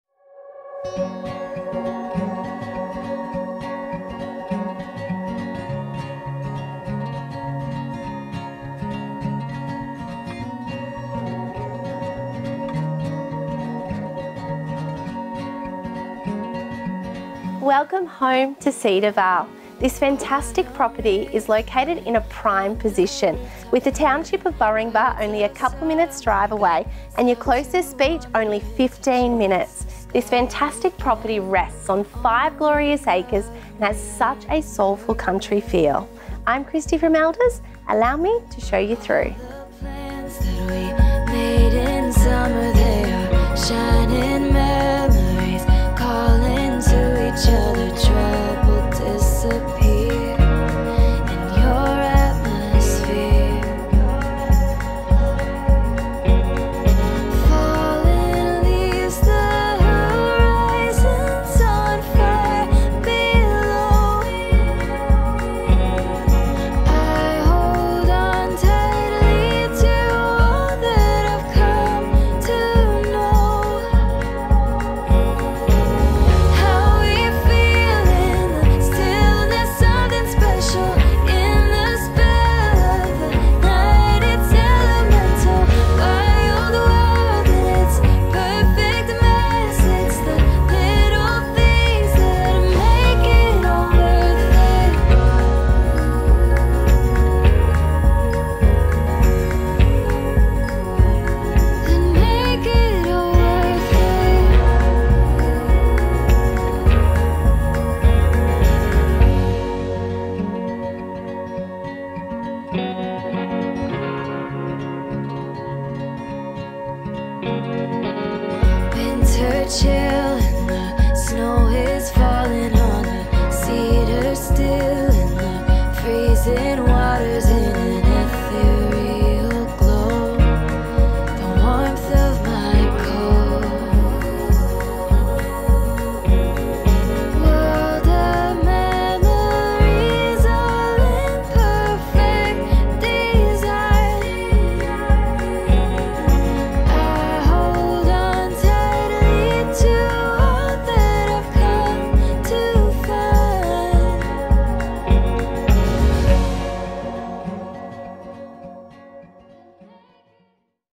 308 Burringbar Road, Upper Burringbar NSW 2483
Welcome to 'Cedarvale'

Circa 1910, this impressive residence has been extended and transformed by the owners of twenty years whilst still retaining its original charm. Set on a generous 2 hectare block with bordering Burringbar Creek and Lychee orchard, the home is enveloped by private leafy surrounds. Please note recent flooding did not affect this flood free property or its 3km access to the quaint village of Burringbar. This property ticks all the boxes for the perfect quintessential country lifestyle.

Features of the property include:

• Four bedrooms all open out to the covered deck including the 46sqm master
• Two bathrooms, one with claw foot bath with shower, the other with shower recess
• Central country style kitchen with timber benchtops and D/W
• The living room also opens to the deck with original fireplace
• Light-filled family/dining with large picture windows
• Spacious entry foyer and central hallway with skylight
• 80sqm of full length wrap around covered deck plus second outdoor area off the family/dining
• LED's and fans throughout
• Separate laundry
• Freshly painted inside and out
• Character features include Rose Gum and Hoop Pine flooring throughout, exposed beams, 3m ceilings, double hung windows and French doors, VJ walls and fretwork
• Over-sized single garage and adjoining carport
• 2 x 35 000 litre water tanks plus 1 x approx. 100 000 litre
• Two irrigation licences and recently relayed sewage
• Fully restumped with stainless steel posts
• The home is surrounded with an array of fruit trees including Mandarin, Mulberry, Mango, some rare rainforest trees, plus Red Cedar, Australian Teak, and Coolamon

Located in the northern hinterland of the North Byron region, Burringbar has fast become a highly sought-after area with hand-crafted cheese from local dairy farms, laden roadside fruit stalls, freshly brewed coffee and Taco nights from Elwood Café and the iconic Heaths Old Wares. Only 13 minutes to the pristine shores of Wooyung Beach and 19 minutes to Pottsville, this truly is a rare opportunity to secure a beautiful home in such a stunning location.

Kristie Hoffman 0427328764 ~ Proudly representing the sale of this property.